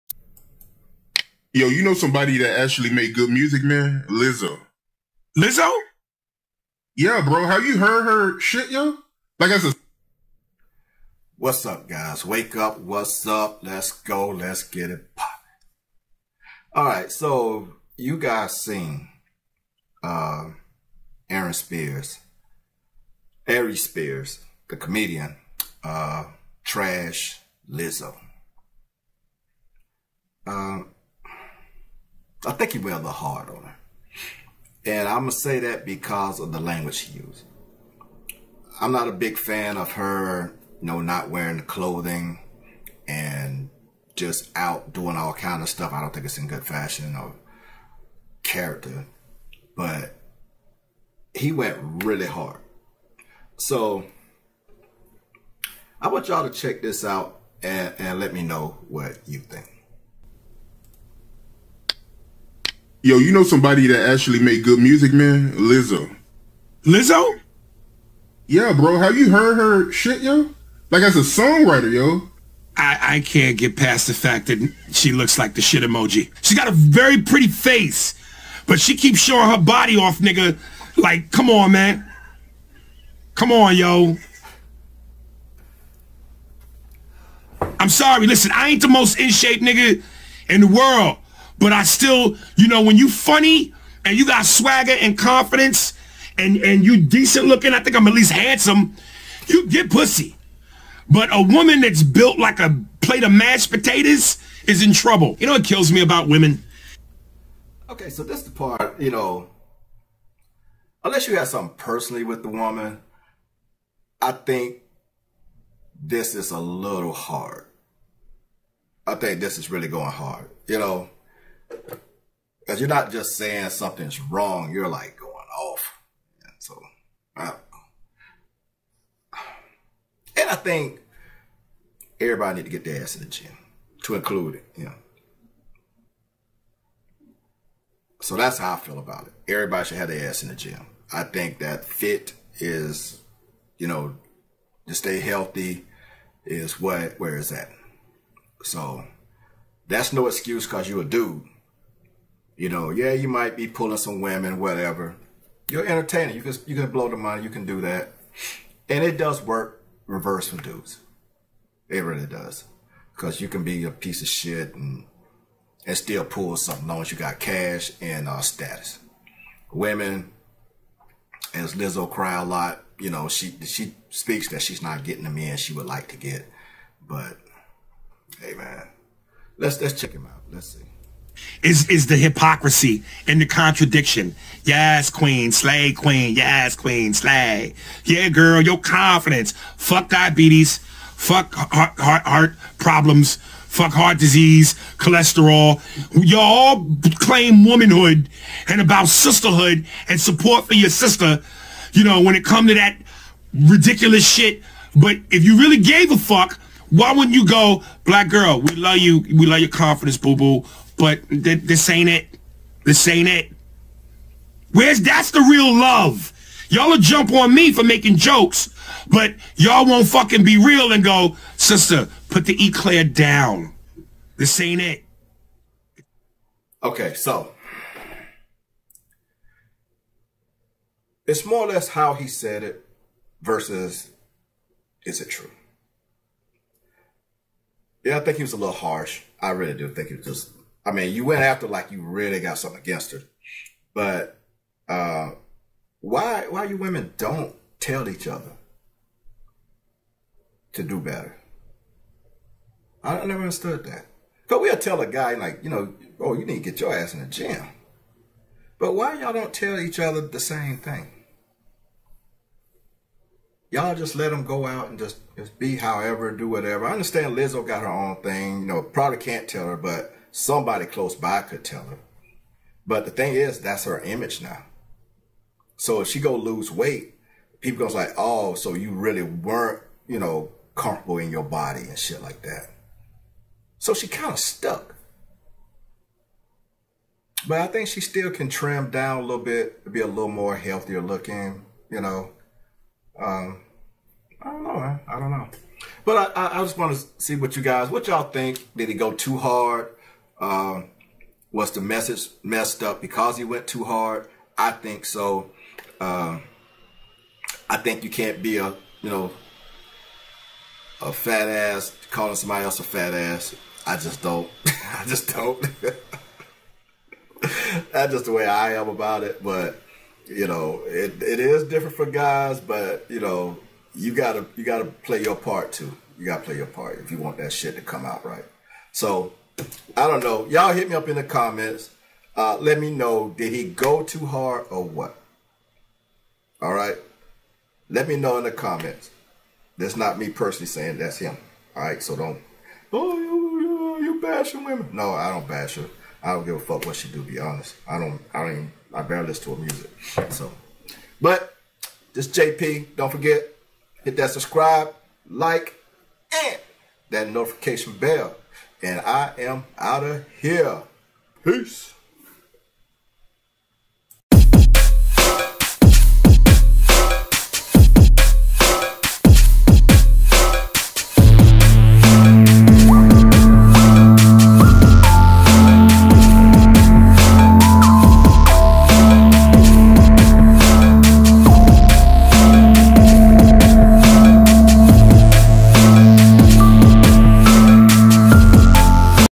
ARIES SPEARS REGRETS CALLING LIZZO THE "SHYT" EMOJI @Lizzo Music
Comedian Aries Spears regrets calling Lizzo the "shyt" emoji.
Aries Spears during an interview insulted @Lizzo for no apparent reason. He decided to go after her appearance when he is overweight himself.
Did he go too far?

My IG or IG: Jp_thesoldier

My Merk link: GET CALM VIBES ATHLETIC WEAR AT:
REEZY CLOTHING LINE, "WHEN COMFORT MEETS DRIP":